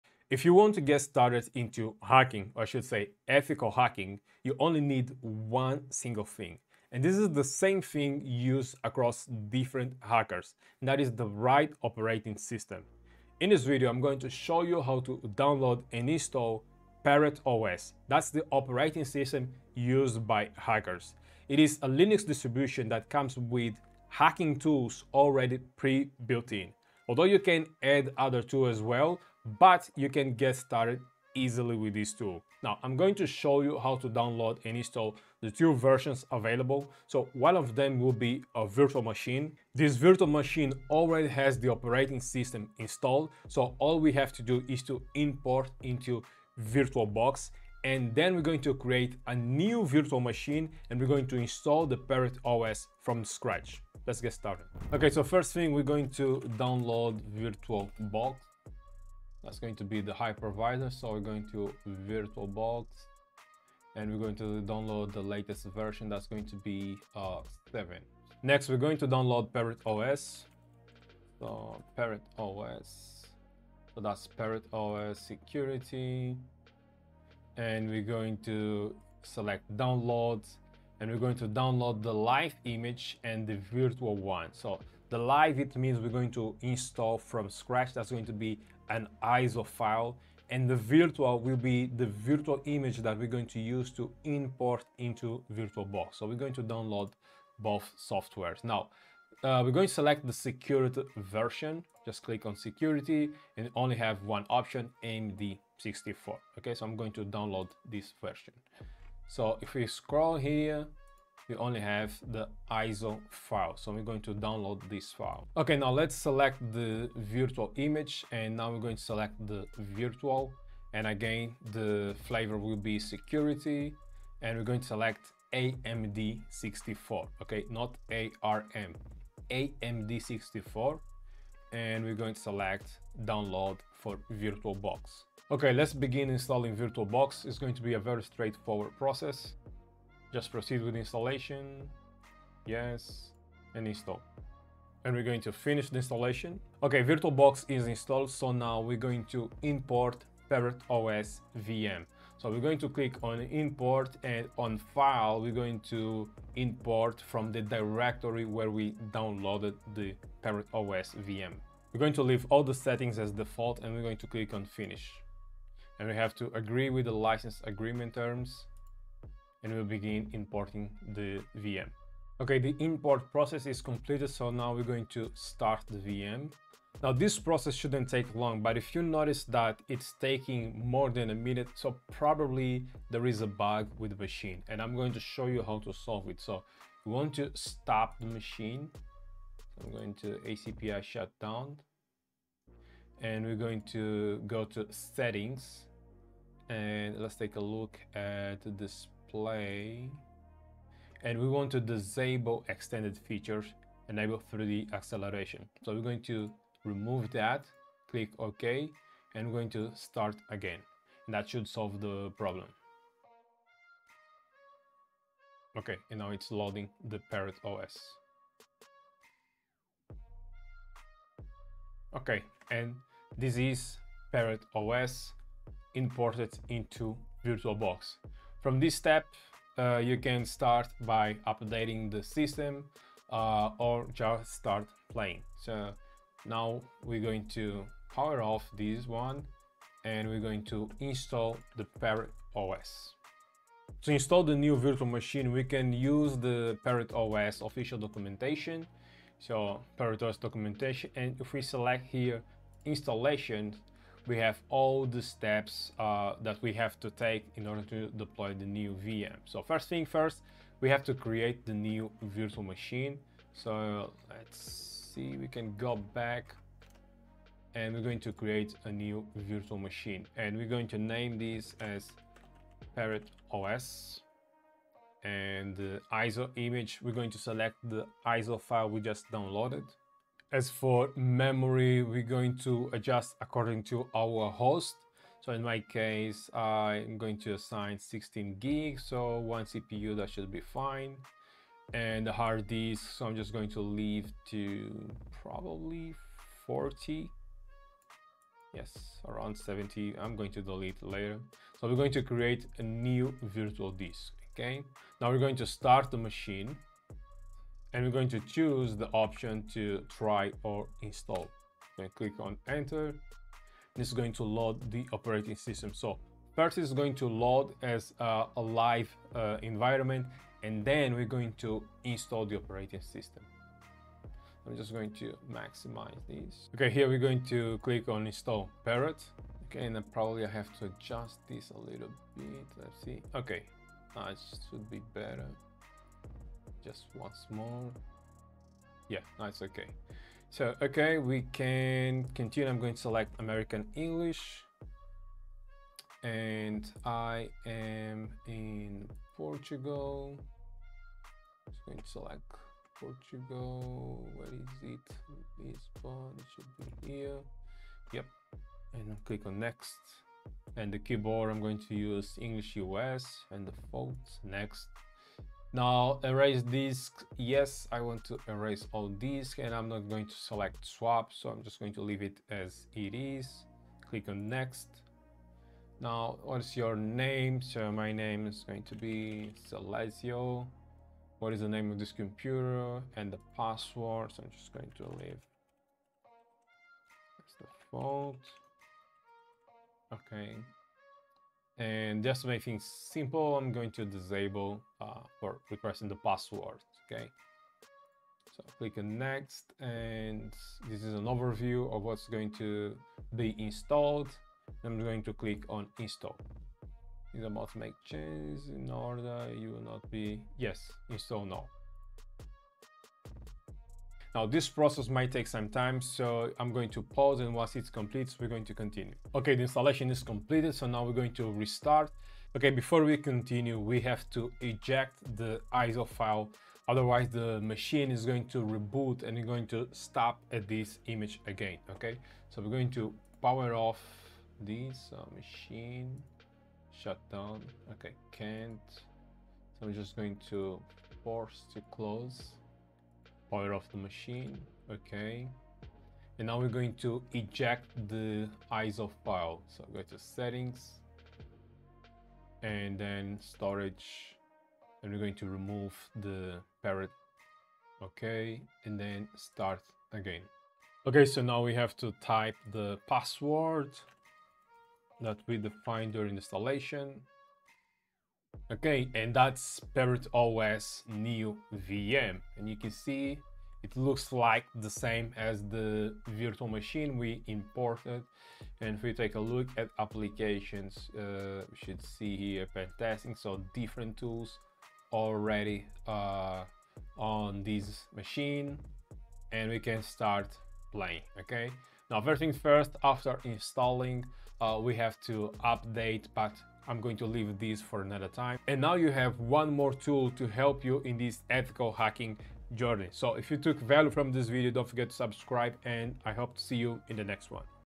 The Operating System Used by Hackers!!
Parrot Security provides a huge arsenal of tools, utilities and libraries that IT and security professionals can use to test and assess the security of their assets in a reliable, compliant and reproducible way.